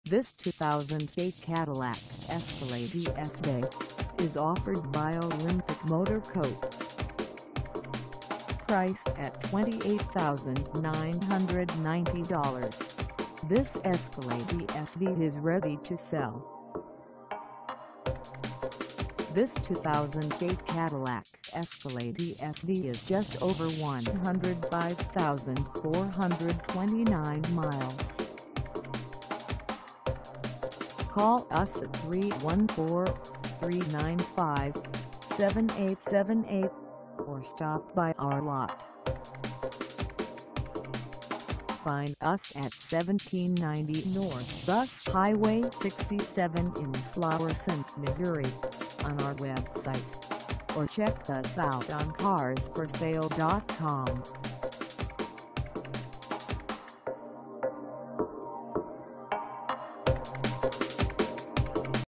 2008 Cadillac Escalade ESV for sale in FLORISSANT, MO 63033
This 2008 Cadillac Escalade ESV is for sale in FLORISSANT, MO 63033 at OLYMPIC MOTOR CO.
Engine: V8 6.2L V8
Transmission: Automatic 6-Speed
Color: Black Raven
Mileage: 105429
Contact OLYMPIC MOTOR CO 1790 N US HIGHWAY 67 FLORISSANT, MO 63033 to test drive this 2008 Cadillac Escalade ESV  today.